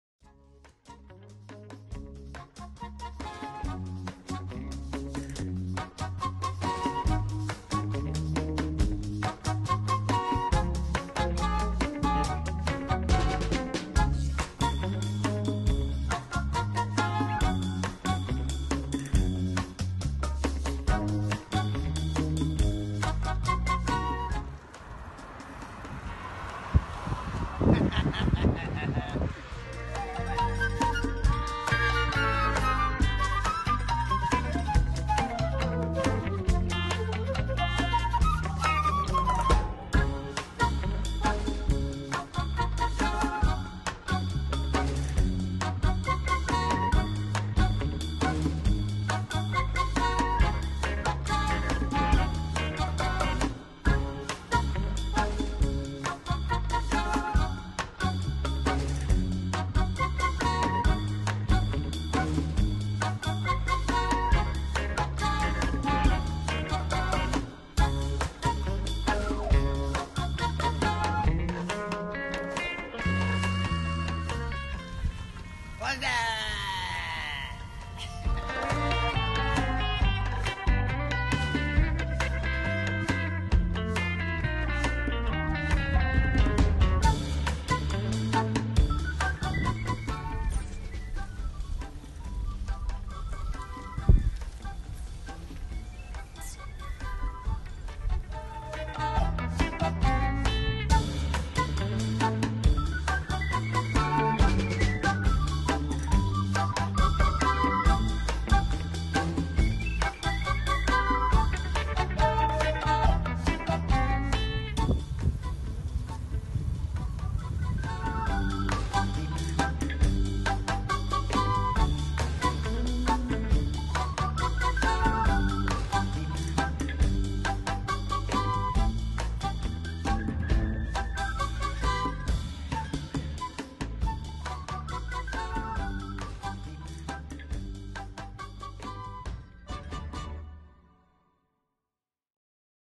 Tongue Out - First biking of 2020
2 of Team Labas Dila cycled from Costco Burnaby to Stanley park. Our First long ride in 2020